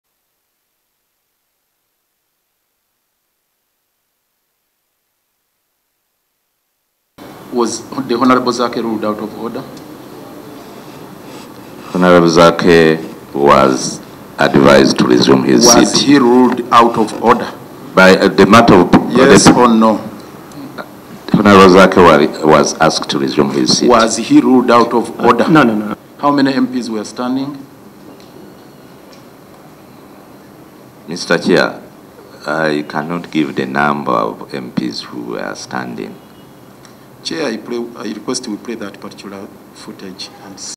Was the Hon. Zaake ruled out of order?
Was the Hon. Zaake ruled out of order? || Kampala Lord mayor Ssalongo Erias Lukwago also lawyer for Mityana Municipality MP Francis Zaake tasks Director of Clerks in Parliament Ignatius Kasirye who was a key witness against   MP Zaake's misconduct before Parliament’s Rules, Discipline, and Privileges Committee.